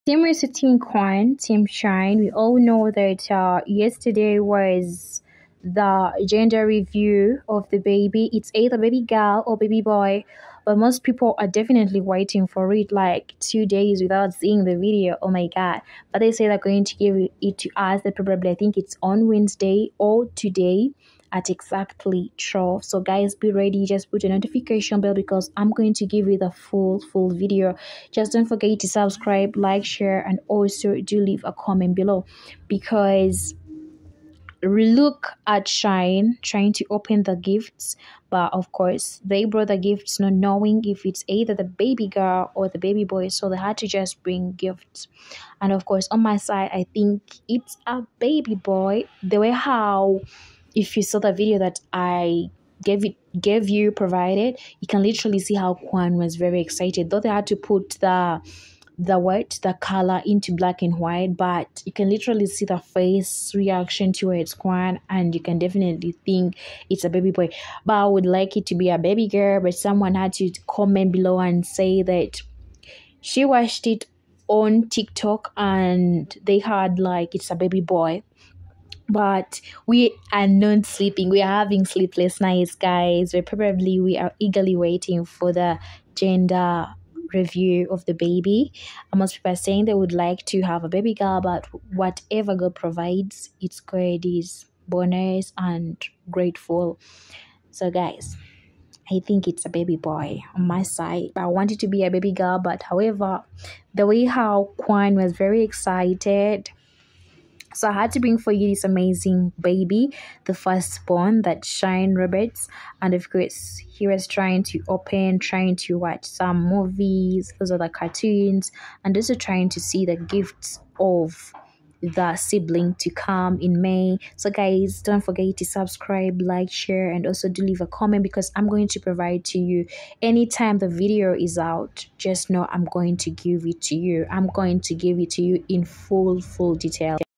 Rissa & Quan Baby Gender Reveal Party Boy Or Girl | Shine Opening The Gifts | Full Details & Video
Congratulations Rissa & Quan